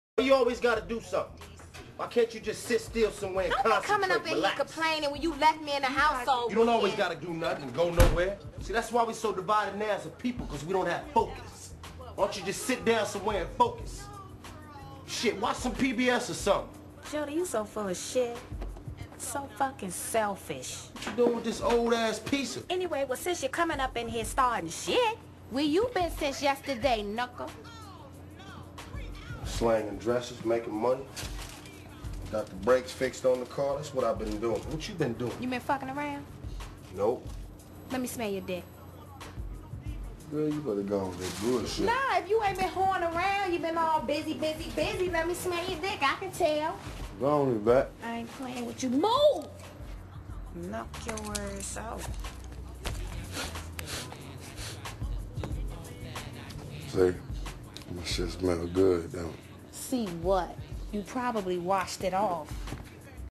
Tyrese v.s Wifey
Tyrese gets into it with wifey